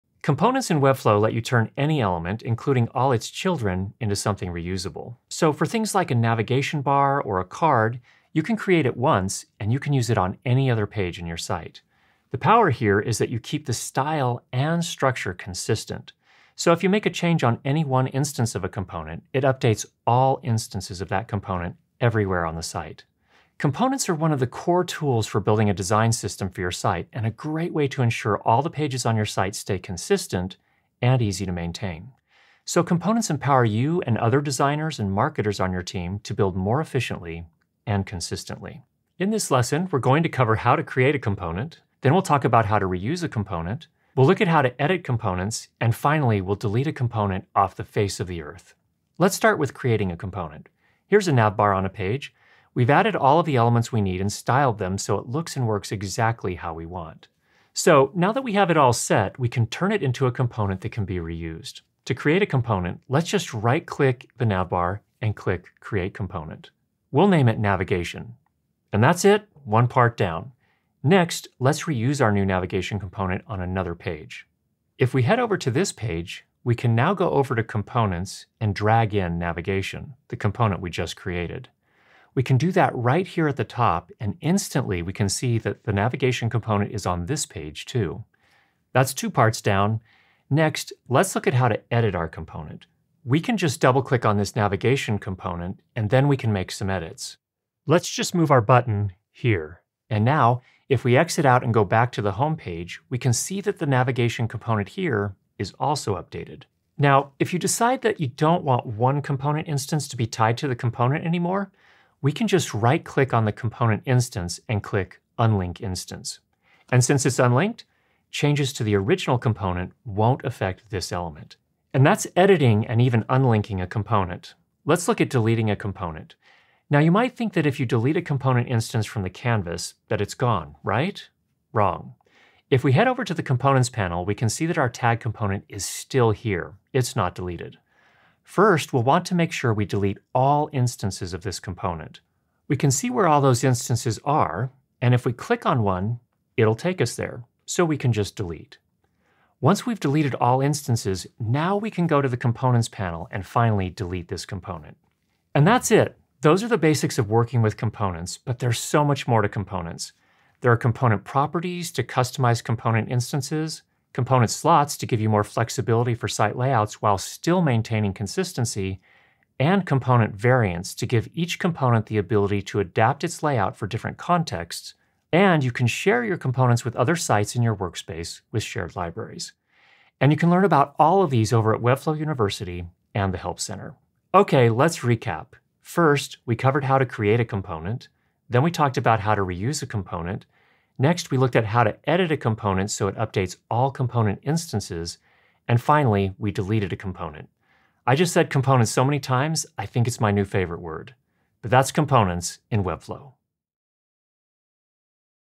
Introduction to Components in Webflow – Webflow tutorial
Components in Webflow empower you to simplify the effort of making your site consistent and easy to maintain and update. Components are one of the main building blocks of a design system. In this tutorial, we show you how to create, reuse, edit, and delete components

00:00 — Introduction
00:49 — Create a Component
01:11 — Reuse a Component
01:28 — Edit a Component
02:04 — Delete a Component
02:36 — Learn more about Component properties, slots, and variants
03:04 — Recap